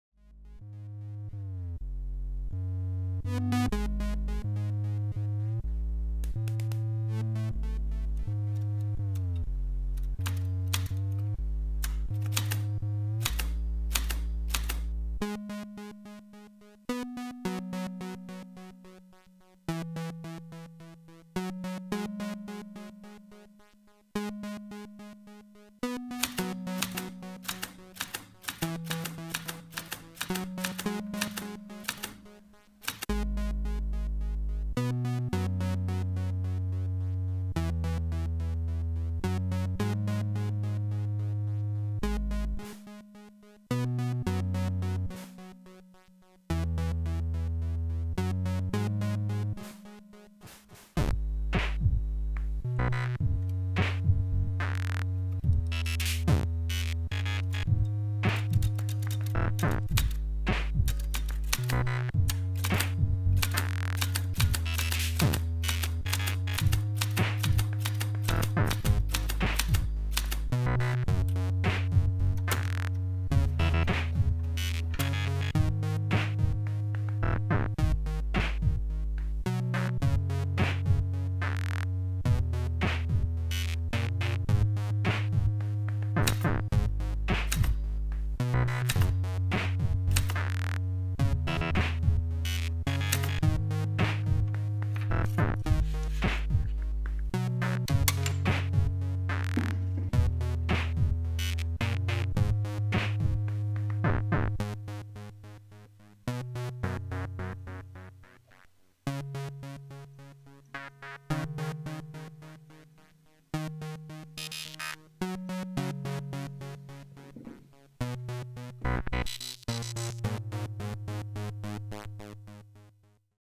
Giant Steampowered Typewriter ( teaser :o) )
Hey, hey ... it's me again with some little teaser for my friends around here on the net. I've started typing the new picture and finally at the end of the day I just threw some vid together including an old Commodore 64 track i once made back in the days. Have fun!